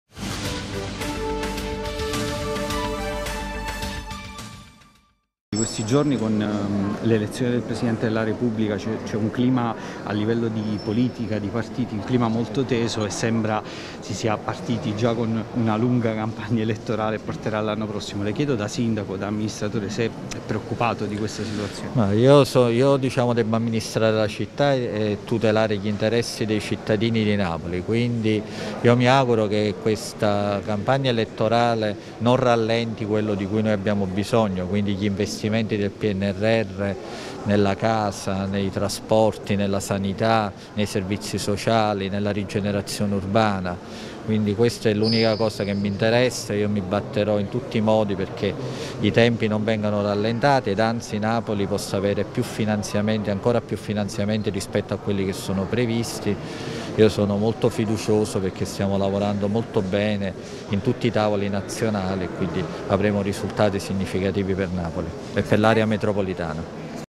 Manfredi, mi batterò per tempi rapidi e nuovi finanziamenti per Napoli
Le dichiarazioni del sindaco Geatano Manfredi sullo scenario politico all'indomani della rielezione di Sergio Mattarella al Quirinale.